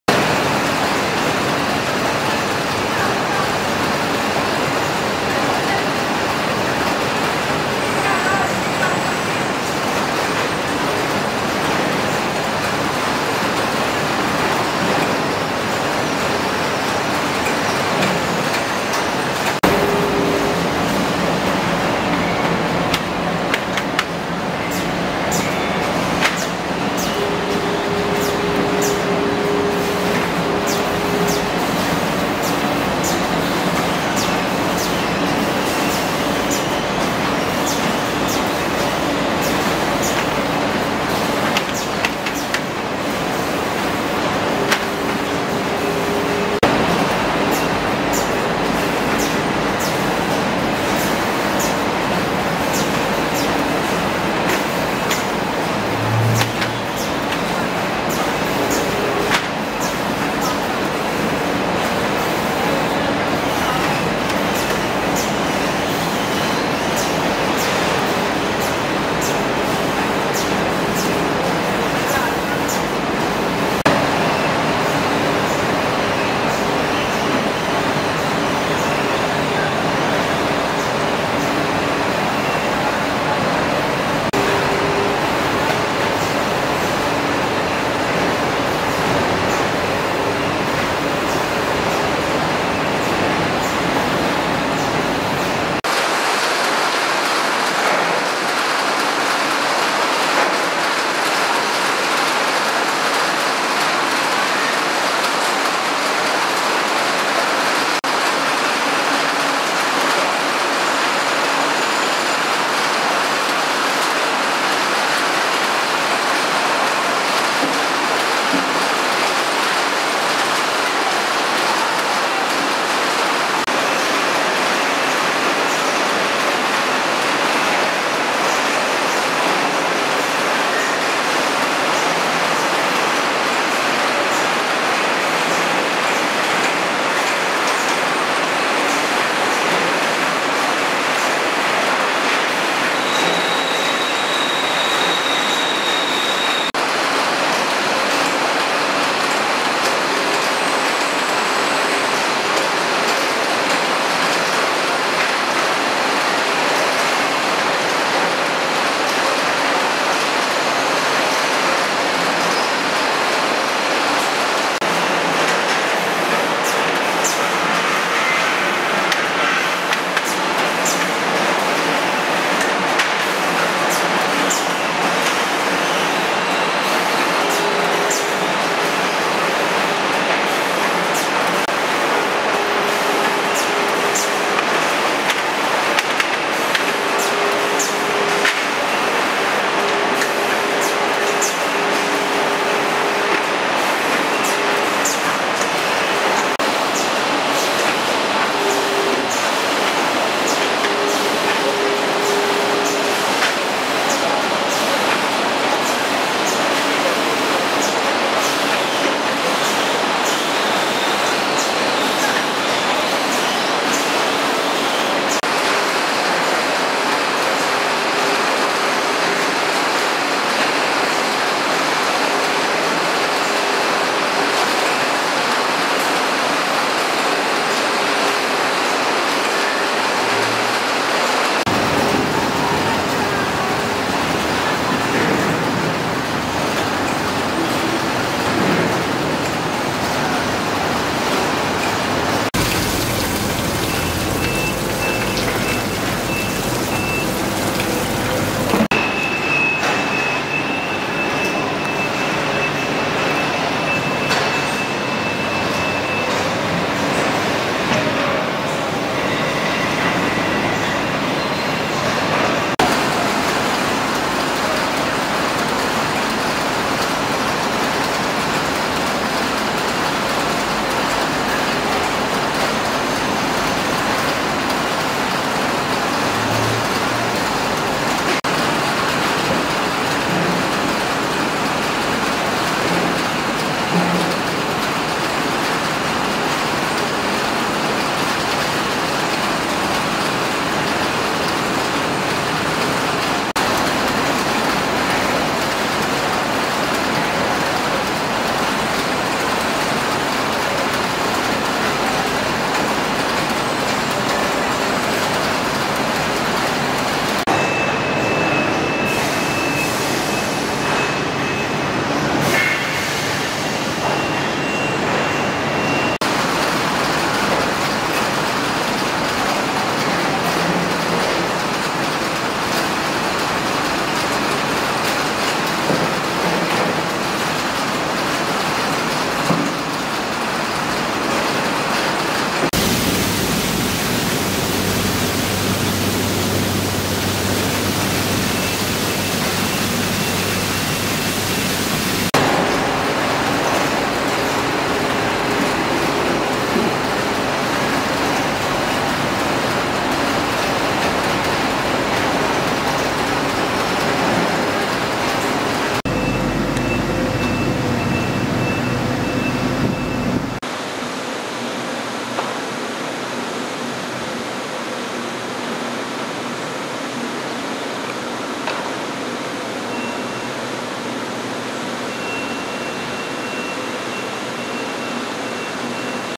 Otkupni centar EnnaFruit koji otkupljuje mandarine od okolnih OPG-ova
Otkupni centar EnnaFruit u Opuzenu koji otkupljuje mandarine od okolnih OPG-ova.